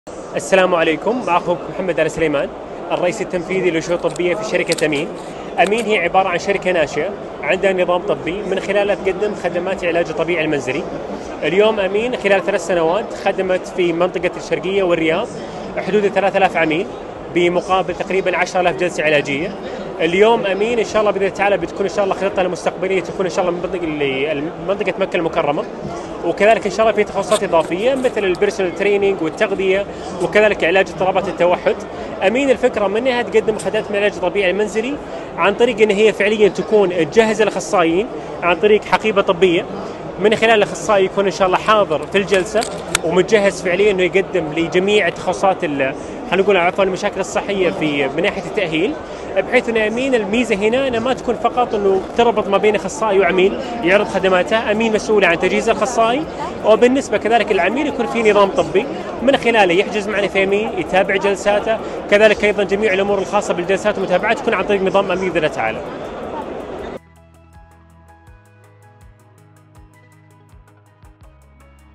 محمد السليمان لـ "مباشر": الشركات الناشئة في المملكة تخدم النظام الطبي المنزلي
الرئيس التنفيذي لشركة أمين الطبية الإستاذ: محمد السليمان
يذكر أن شركة أمين الطبية من الشركات الناشئة في المملكة تخدم النظام الطبي المنزلي ، وصل إجمالي الخدمة حوالي 3000 شخص منذ بدايته حتى اليوم ، واشار الى التطلعات التطويرية المستقبلية